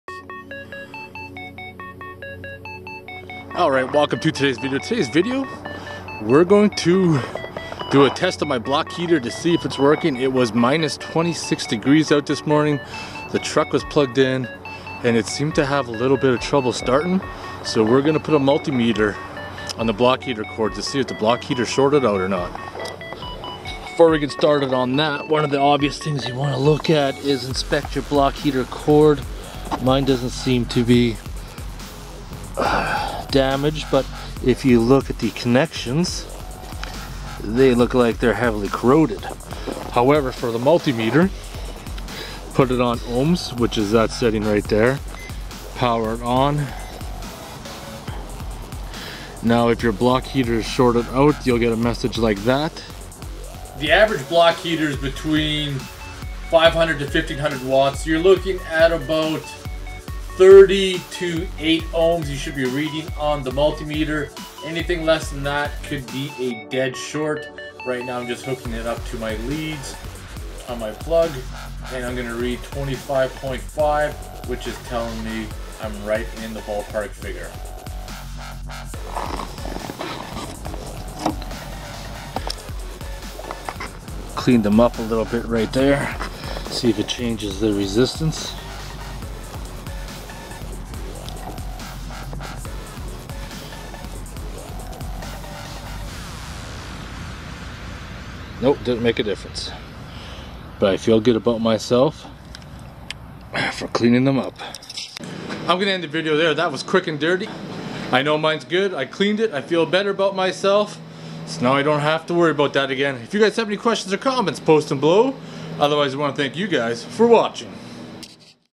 How to Check Your Block Heater
Winter is here and one of the most important things you need to make sure that works is your block heater.  inspect the cord for damage and also clean the connections if they are corroded.  the Average vehicle block heater is about 500-1500 watts which should equal to about 8-30 ohms that you will read with a multimeter.  Anything less than 8 ohms can indicate a short and any high resistance can result in the block heater getting ready to fail